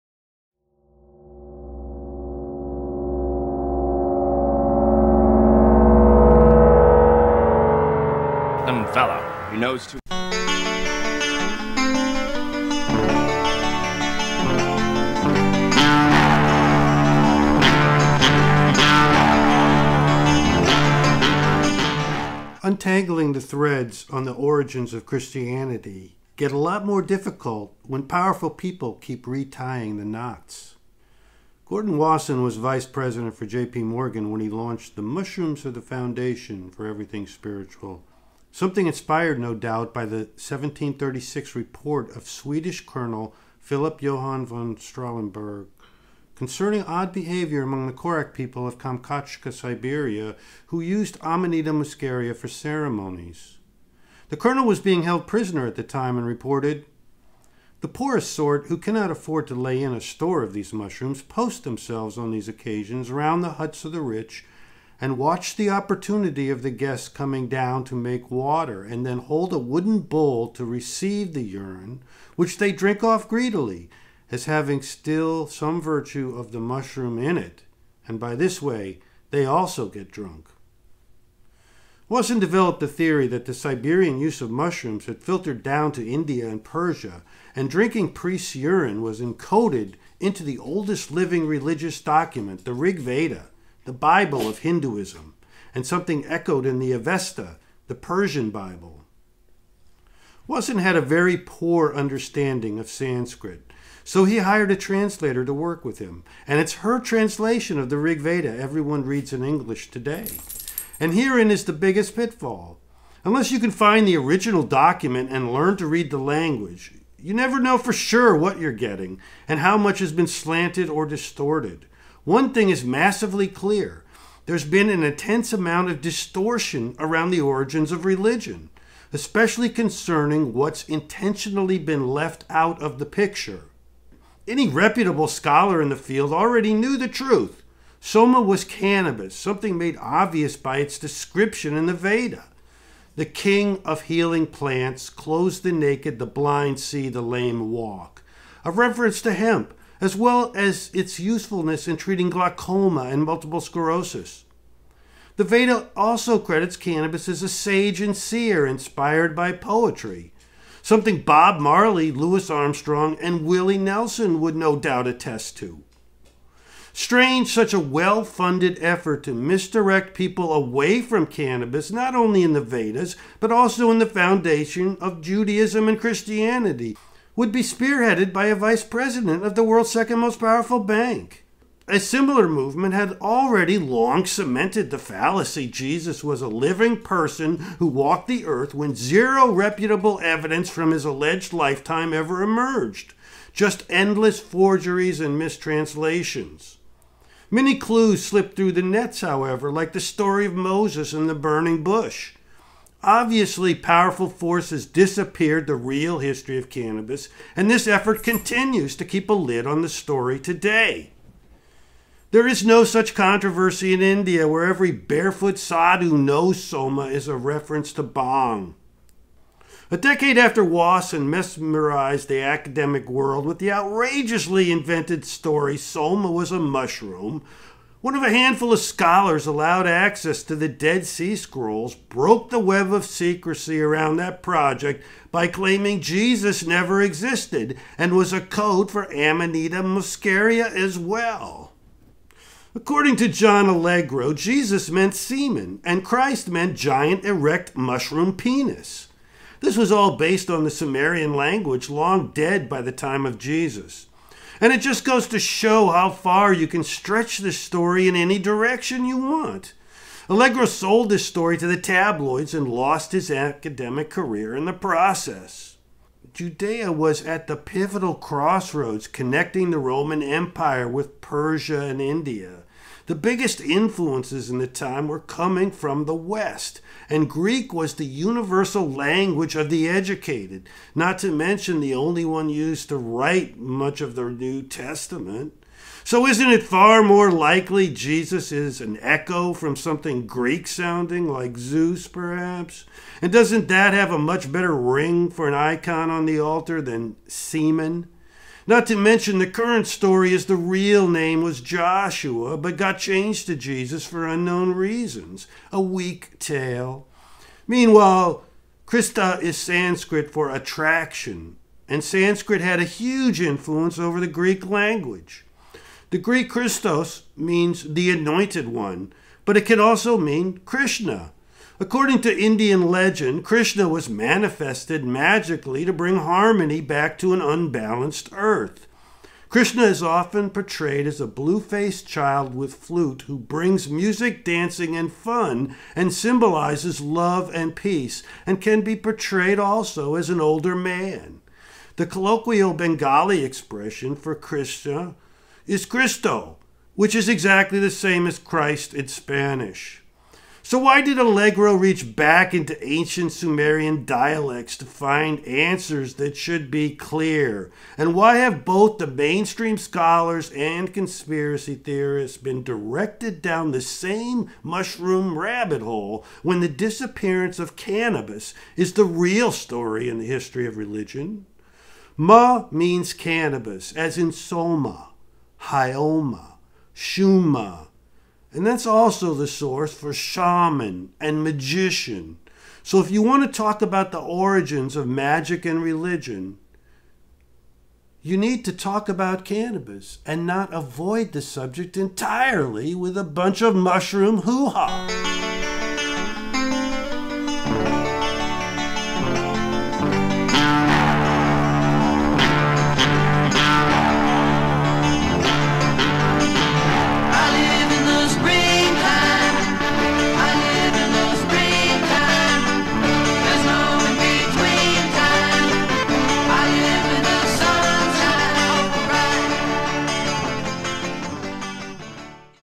Origins of the mushroom conspiracy theory
Why have powerful forces conspired to obscure the real history of religion?